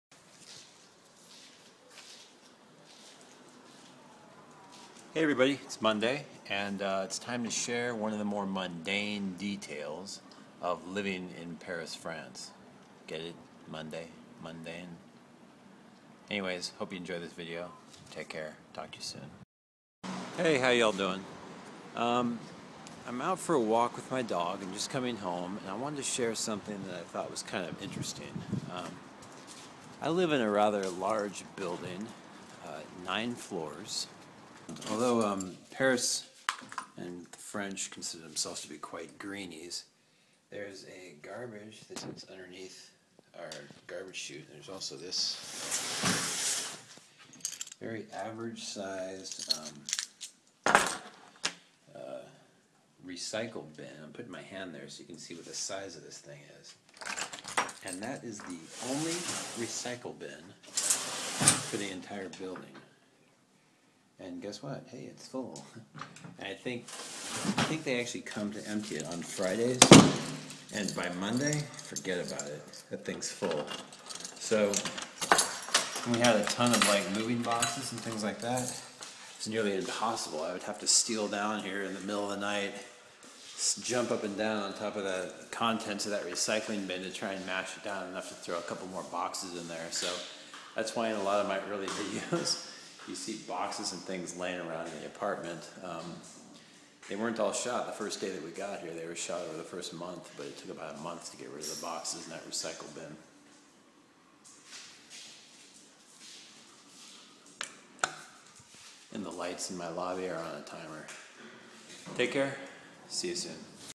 Paris VLOG - Mundane Monday - Recycling Bins
Just showing you some more of everday living in France...Yes, it's true, it's not all sitting in streetside cafes, sipping espressos and pontficating about the class struggles with other artists. You actually have to do boring everyday stuff like laundry and taking out the trash when you live here